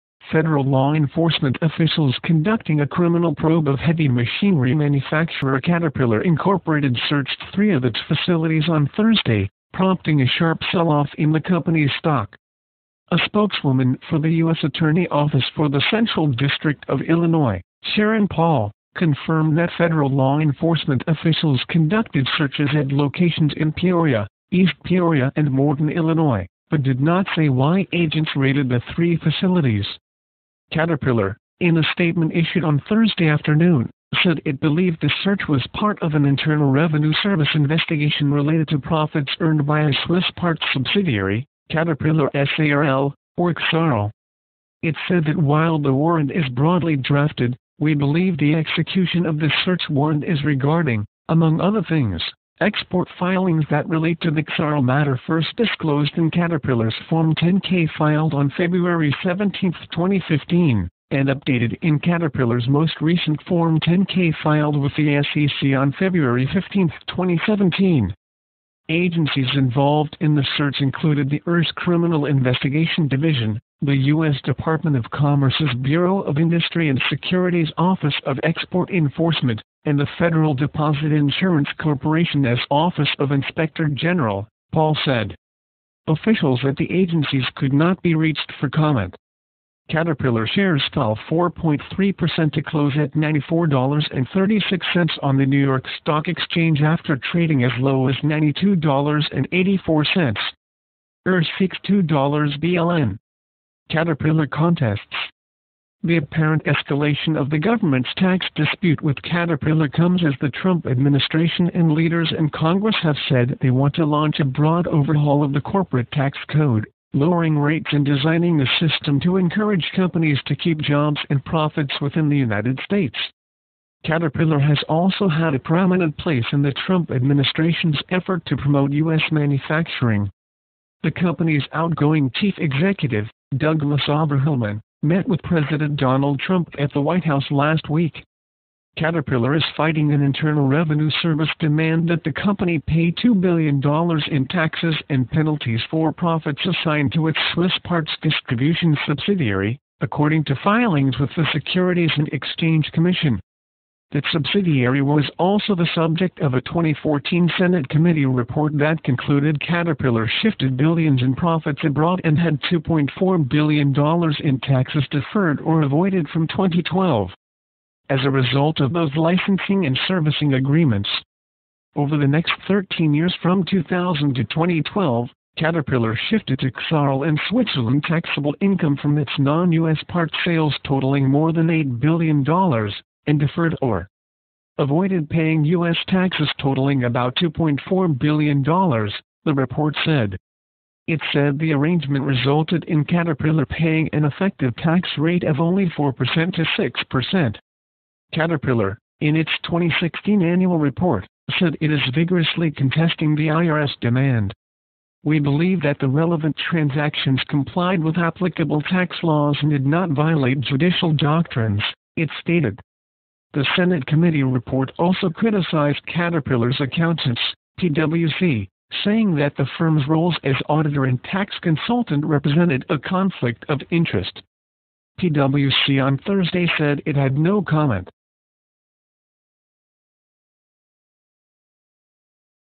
Years of federal scrutiny lead to Caterpillar HQ raid | hungry caterpillar | very hungry caterpillar
Years of federal scrutiny lead to Caterpillar HQ raid | the very hungry caterpillar | caterpillar stock | gaudy sphinx caterpillar

Years of federal scrutiny over Caterpillar's overseas business practices and tax strategies came to a head Thursday with a raid on the heavy machine maker's Peoria headquarters.

Authorities from three agencies, including the Internal Revenue Service, arrived before 11 a.m. at the headquarters and two nearby facilities. Initially only confirming the raid, Caterpillar later in the day acknowledged it was tied to the same issue that has dogged the company for eight years: its use of a parts subsidiary based in Switzerland and tax-saving practices that sparked a Senate investigation, shareholder lawsuits and a $1 billion penalty.